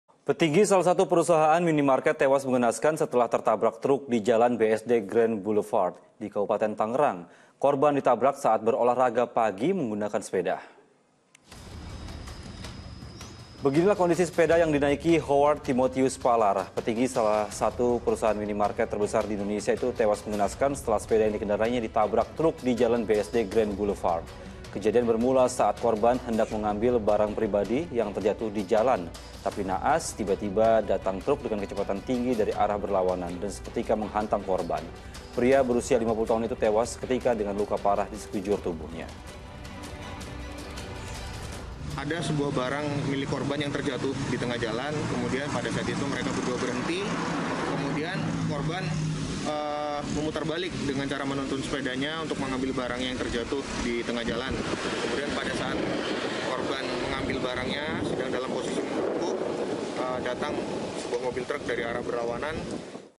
Bos Indomaret Tewas Tertabrak Truk Saat Bersepeda | Patroli
Petinggi salah satu perusahaan minimarket tewas mengenaskan setelah tertabrak truk di Jalan BSD Grand Boulevard, Kab. Tangerang. Korban ditabrak saat berolahraga pagi menggunakan sepeda.